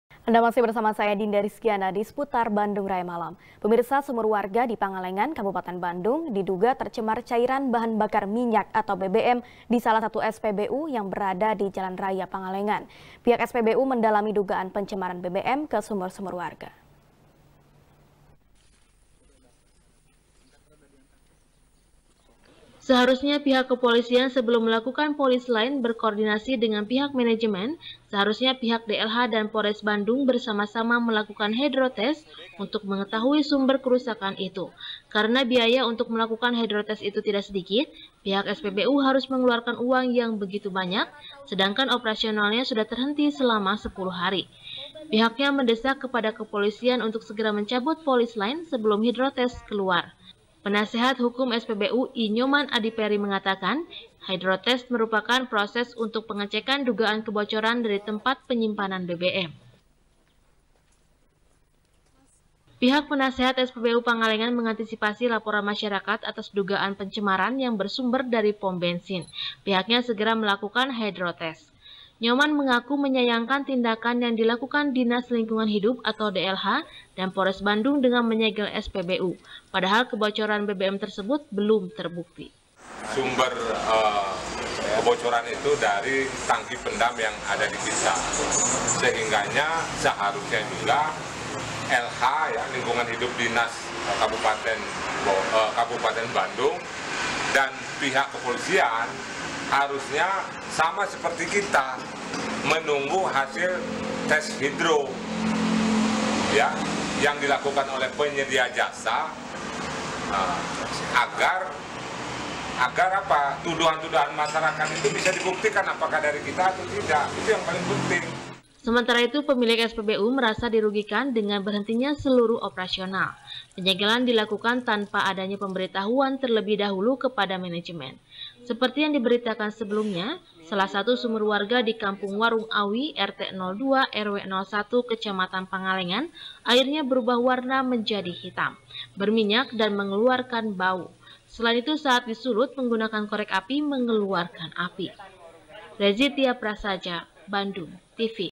Kuasa Hukum Pertanyakan Mekanisme Penyegelan SPBU oleh Polres Bandung | SBR | BANDUNGTV
SBR | BANDUNGTV | DINDA RIZKYANA | Sumur warga di Pangalengan, kabupaten Bandung, diduga tercemar cairan bahan bakar minyak atau bbm di salah satu spbu yang berada di jalan raya Pangalengan. Pihak SPBU mendalami dugaan pencemaran bbm ke sumur-sumur warga.